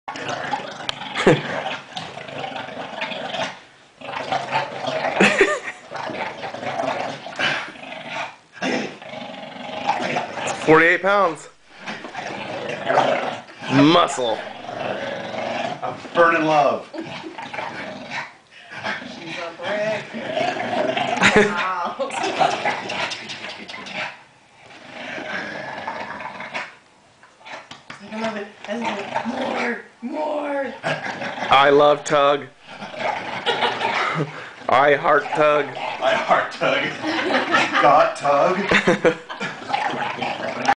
Darla tugs with Andy!
My bullie plays with my Cousin Andy. Tugging is fun.